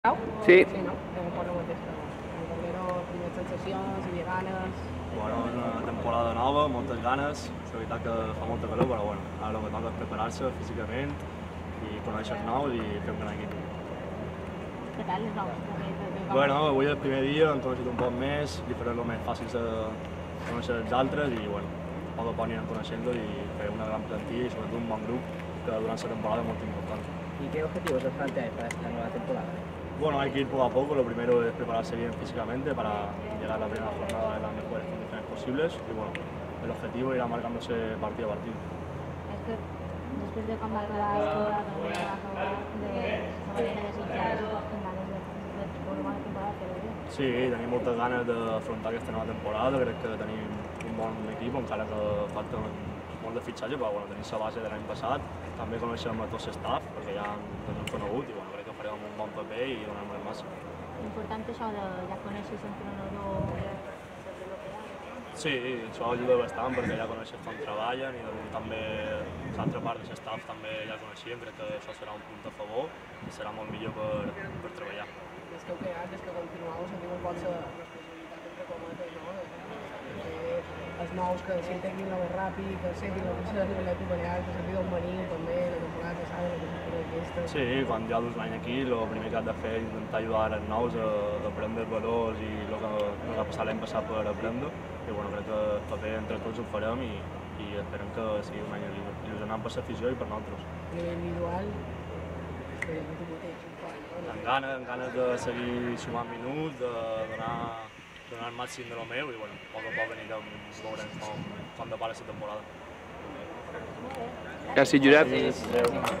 Atlético Baleares | Declaraciones de Josep Jaume primer entreno de la pretemporada 22/23
Josep Jaume: "Es una temporada nueva, con muchas ganas, hace mucho calor pero toca prepararse físicamente y conocer a los nuevos. Queremos hacerles fácil su integración para hacer un buen grupo, que eso durante la temporada es importante. Hay que ir poco a poco, ahora a prepararse físicamente y el objetivo irá marcándose partido a partido. Tenemos muchas ganas de afrontar la temporada, tenemos buen equipo, la base del año pasado aunque queden fichajes y conocemos todo el staff técnico. Ayuda mucho saber cómo trabaja el cuerpo técnico y es un punto a favor para trabajar. Cuando llevas un año aquí toca enseñar los valores del club a los nuevos y lo que pasó el año pasado, es un año ilusionante. Con ganas de dar lo mejor de mí y sumar muchos minutos".